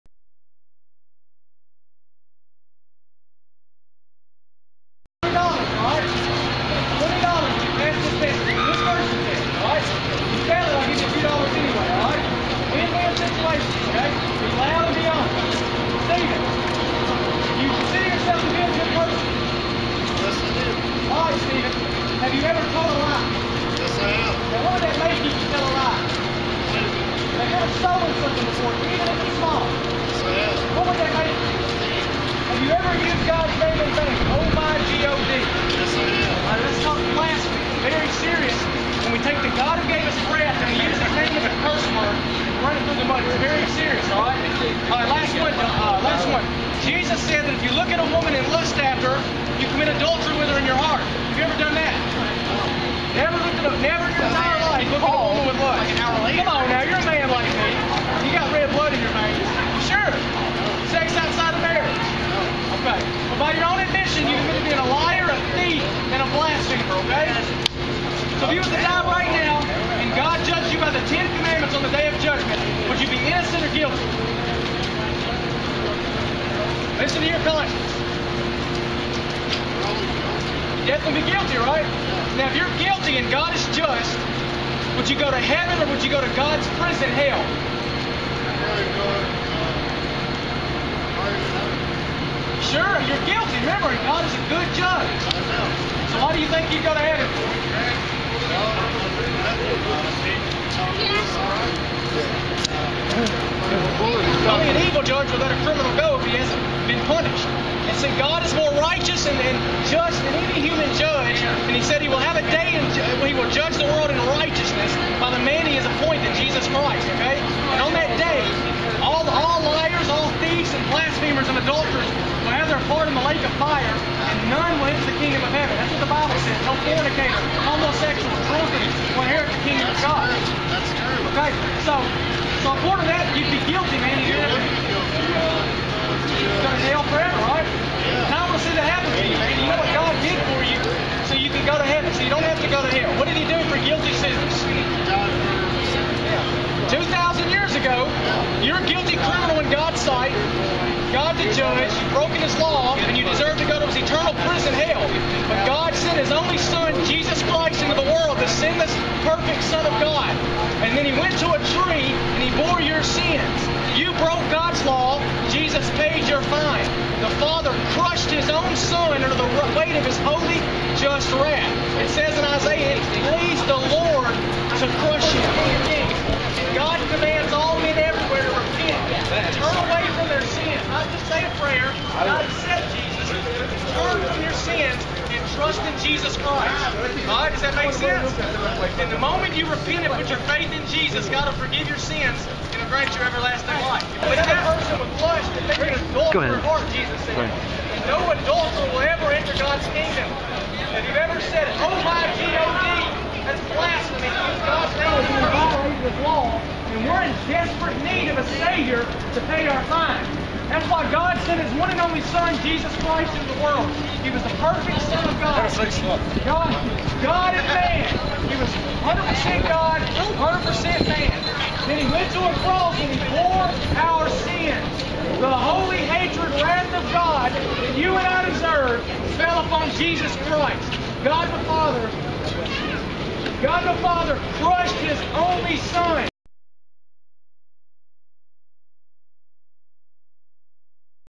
Jamie Terry - Open Air Preaching
Jamie preaching on the pedestrian bridge in downtown Nashville before a Titans football game.  - Dec. 2013